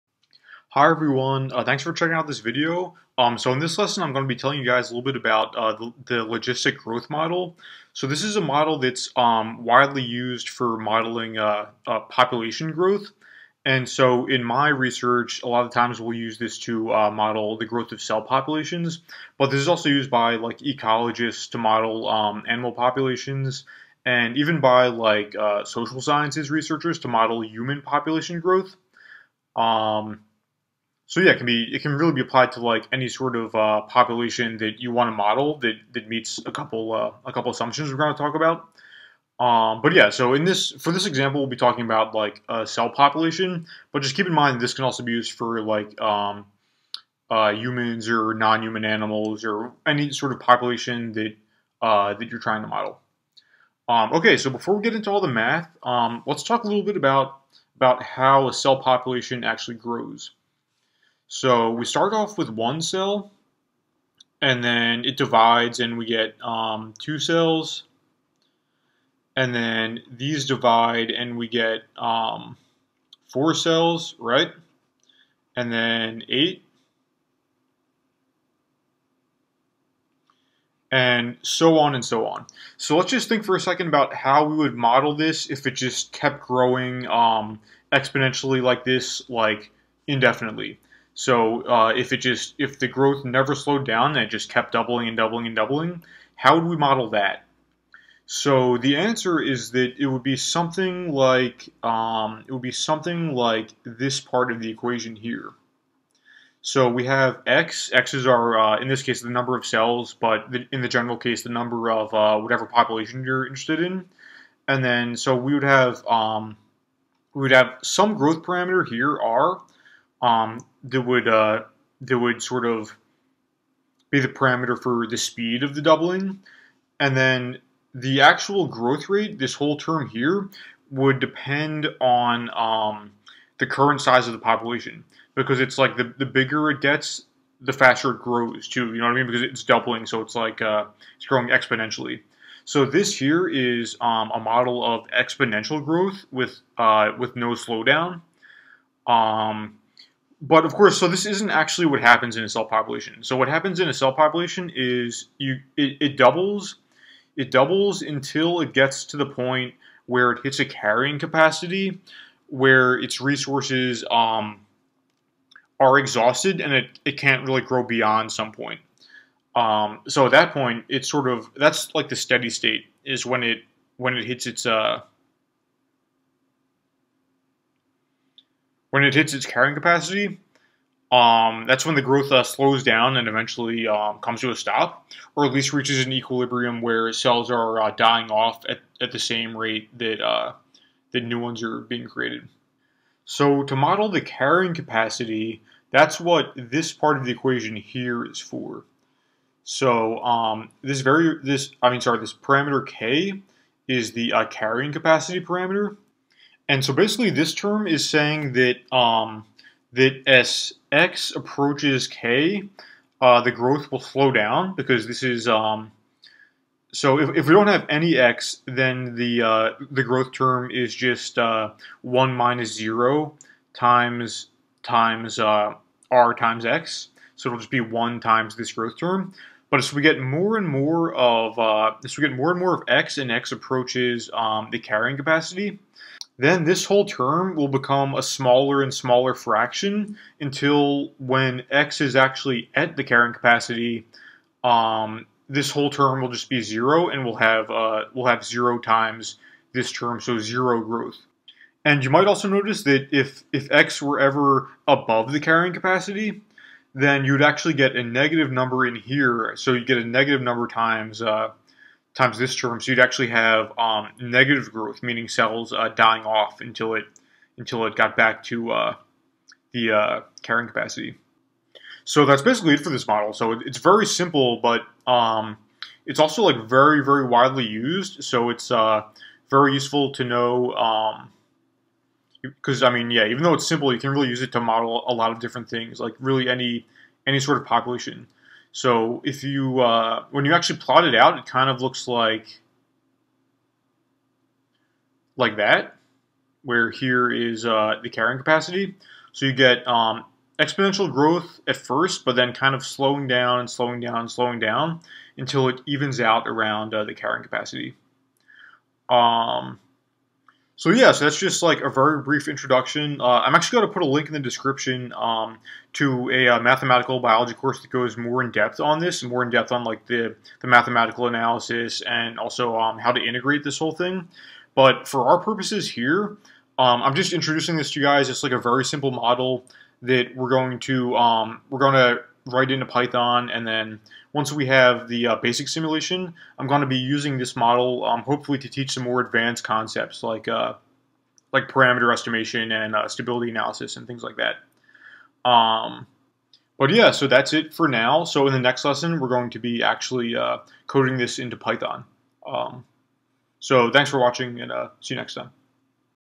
Logistic Growth Model Overview
Hi everyone! This video gives a brief overview of the logistic growth model, which is used for modeling the growth of populations that have a carrying capacity.

This is a great course taught by German Enciso that goes more in depth on the math analysis of the logistic growth model-

If you're are trying to catch up on the basics of Python, differential equations, or cell biology, here are some good resources: